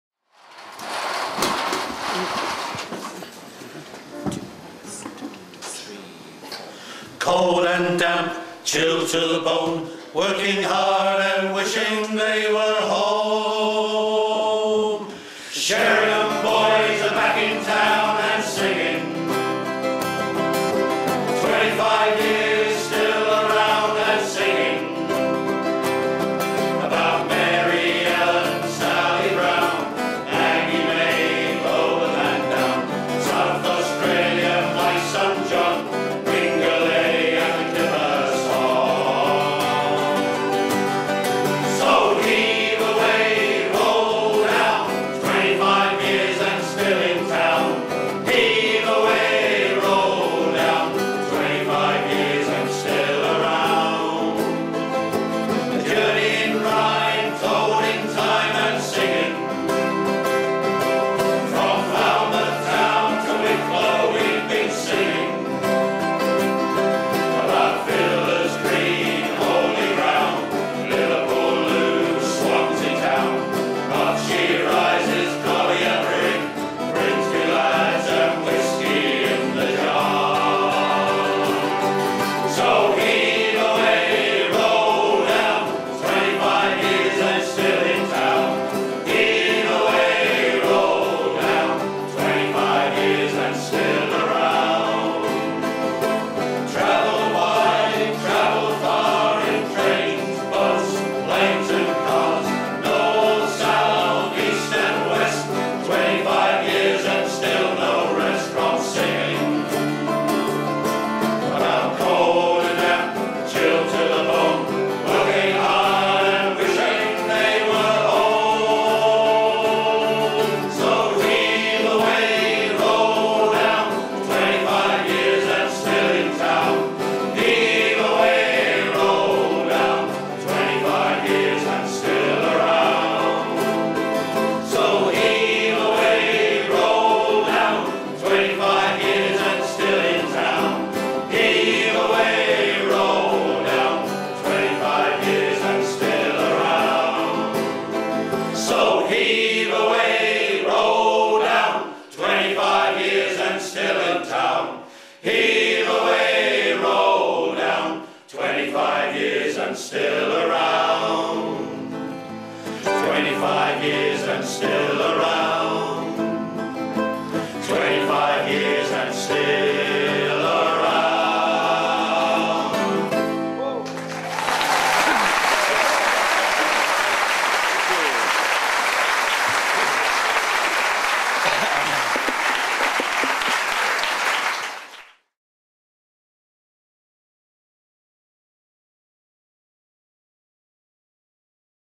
Sheringham Shantymen - 25 Years Still Singing
6th November 2015 saw the 25th anniversary concert of the Sheringham Shantymen, one of the UK's leading shanty choirs. The full selection of songs from the concert will be coming out on DVD but we thought you'd like a taster of the first number - together with some inserts from videos of the Shantymen from the past quarter century.